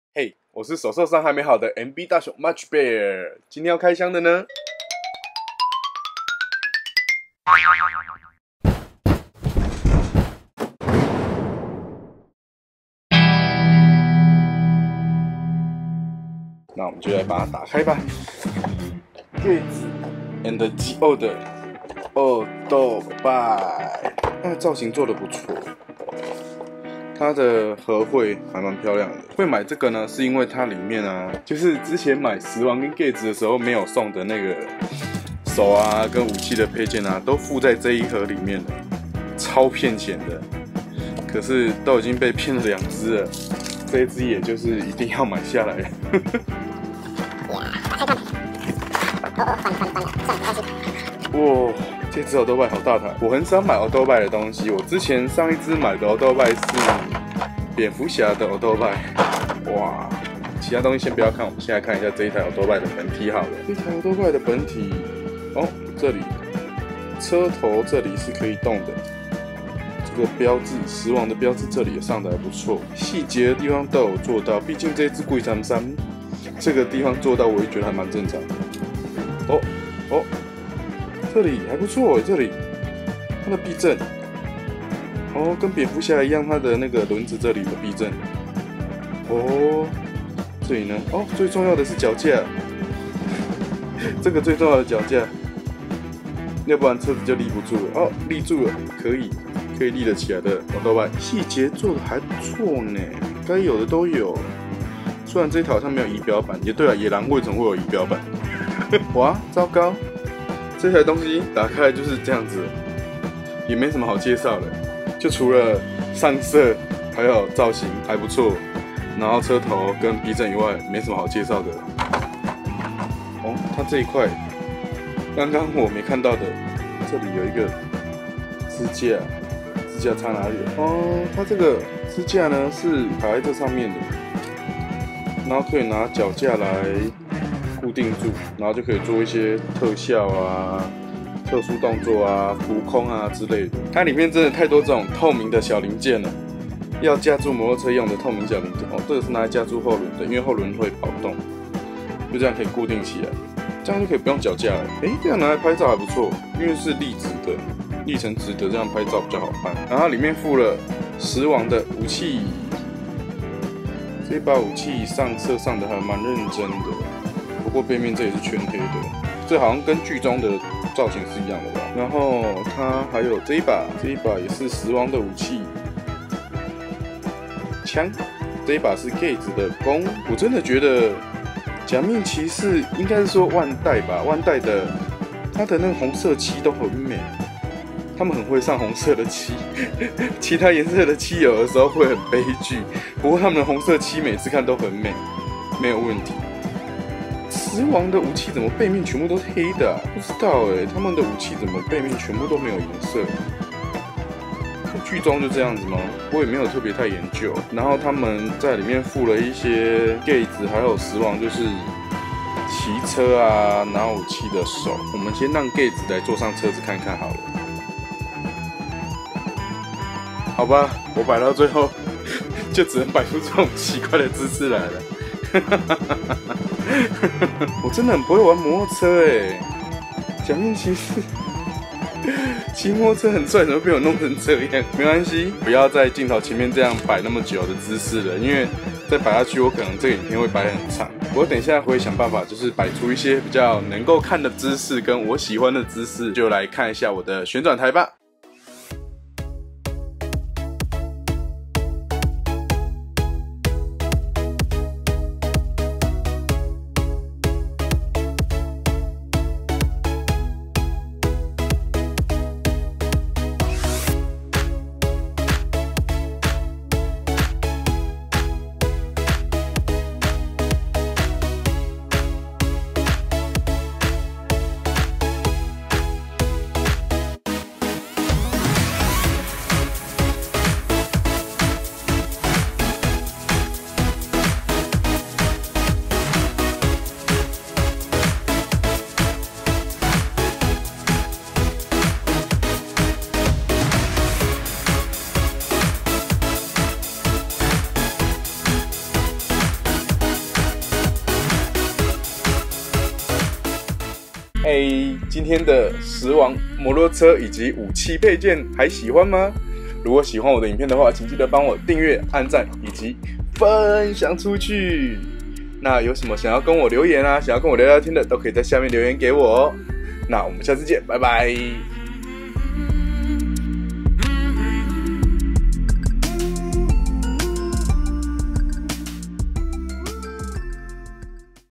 『MB大熊開箱』假面騎士時王ZI-O機車武器組終於來了！！#31
時王 假面騎士 機車 ZI-O
之前開箱的兩隻騎士除了基本手掌以外完全沒有武器
全都在這台機車裡面啦！超燒！
可是身為假面騎士愛好者一定要給他買下手！
還好整體都非常棒沒有讓人失望

有什麼建議或是想跟大熊說甚麼都可以留言喔～
我會回應的～